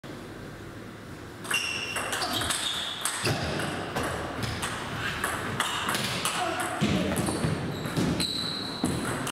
Копия видео "26 ноября 2018. Синий зал. Вечерний турнир. TT Cup"
Катков И. - Дидух В.
Катков И. - Когут Б.
Игра за 1 место Катков - Лимонов
Приглашаем Вас посетить наш канал TT Team для просмотра матчей регулярного кубка "TT Cup" по настольному теннису в онлайн режиме
Живите настольным теннисом вместе с нами